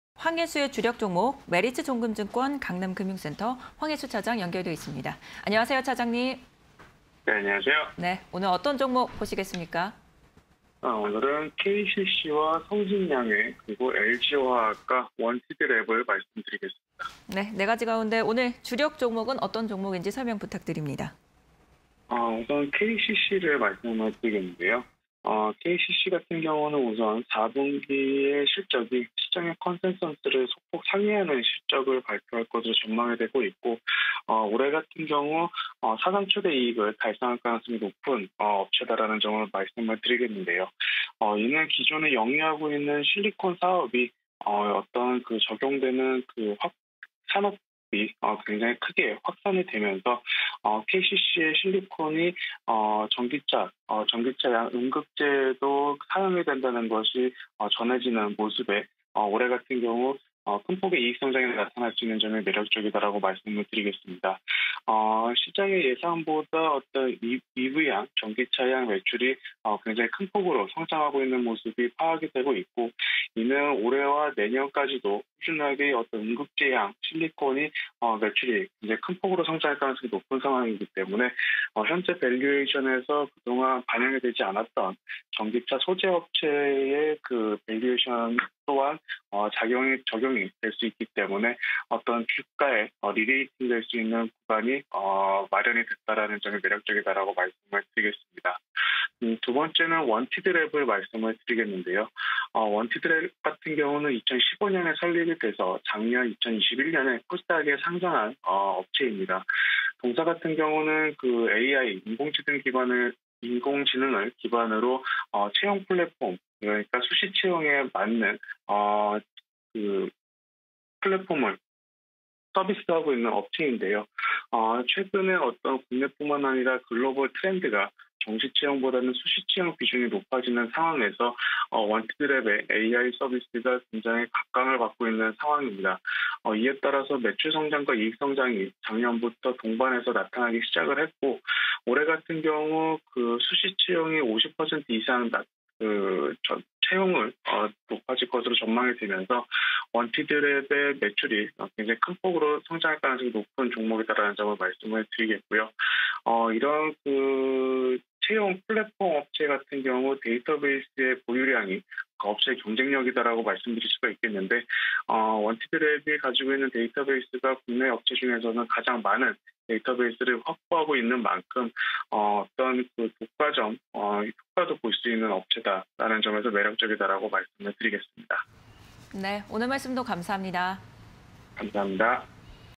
KCC, 원티드랩, 성신양회, LG화학_황해수의 주력 종목 (20220106)
1. KCC(002380)
2. 성신양회(004980)
3. LG화학(051910)
4. 원티드랩(376980)

KCC(002380)
- 4분기 컨센서스 상회 실적 전망
- 실리콘 적용 산업 확대에 따른 성장 원년
- 시장 예상보다 높은 EV향 매출

원티드랩(376980)
- 우호적 환경 조성으로 높은 수혜 예상
- 동종업종 內 가장 많은 DB
- 매칭 채용 시장 개화에 따른 이익 성장